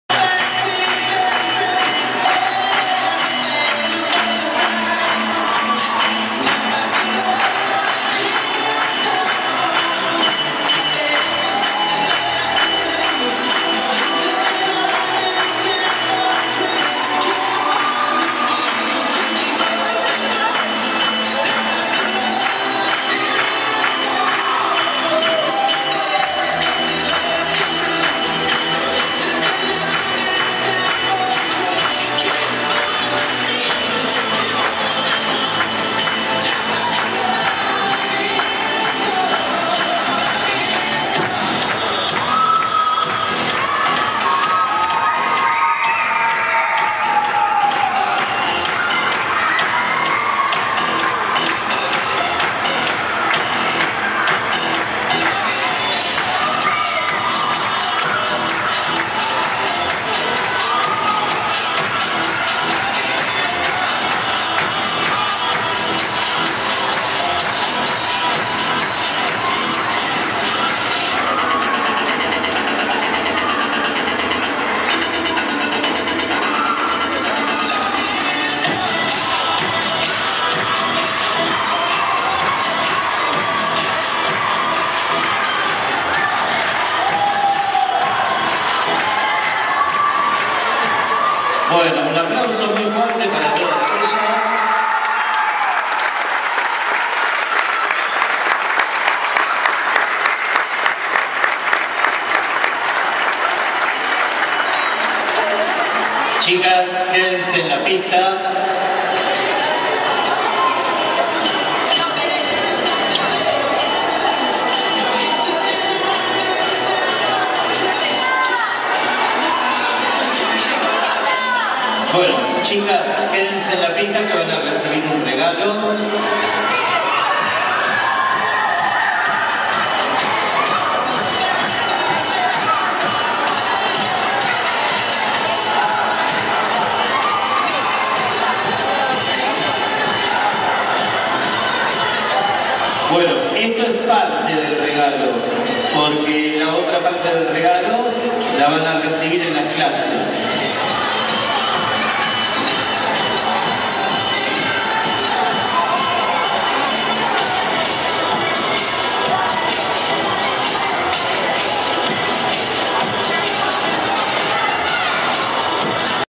Salida final coreo,todos los niveles!!! 2011.MP4
Salida final todos los niveles coreografia noviembre 2011 Megatlon All Boys,Floresta,Bs.As.,ARGENTINA